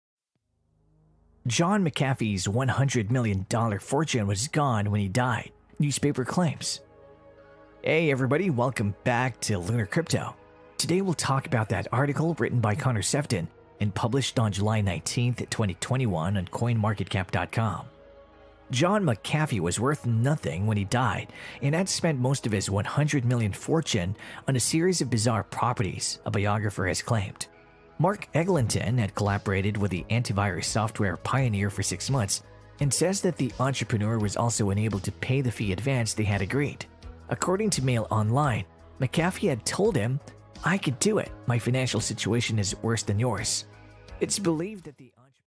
John McAfee's $100M Fortune Was Gone When He Died, Newspaper Claims
Coinbase is one of the best, safest and easiest places to buy and sell cryptocurrency.

Written By: Connor Sephton

John McAfee was worth nothing when he died and had spent most of his $100 million fortune on a series of “bizarre” properties, a biographer has claimed.

Mark Eglinton had collaborated with the antivirus software pioneer for six months — and says that the entrepreneur was also unable to pay the fee advance they had agreed.

According to Mail Online, McAfee had told him:

“I can’t do it, my financial situation is worse than yours.”
It is believed that the entrepreneur owned properties in Belize, Colorado, Hawaii, Texas, Colorado and Tennessee — but ended up having to sell them at a sizeable loss when the recession hit.

One compound that had been valued at $25 million ended up selling at an auction for just $5.8 million.

Eglinton’s claims would chime with one of the last tweets that McAfee wrote, which said:

“The US believes I have hidden crypto. I wish I did but it has dissolved through the many hands of Team McAfee (your belief is not required), and my remaining assets are all seized. My friends evaporated through fear of association. I have nothing. Yet, I regret nothing.”

Janice McAfee Reacts

McAfee was found dead in his Spanish jail cell — hours after a court there ruled that he could be extradited to the U.S. to face tax evasion charges.

Although a post-mortem examination has suggested that he killed himself, his wife Janice has cast doubt on this.

Reacting to the Daily Mail’s report, she tweeted:

“John spoke about being broke numerous times in interviews as well as tweeting about it. This was not John's first time being without money & he was not suicidal because of it. Actually it was quite the opposite. He never felt more free.”

On Friday, she also confirmed that she has received the police report concerning the events surrounding her husband’s death, adding:

“Once the lawyers and myself have had a chance to go over them I will share with you all what I can.”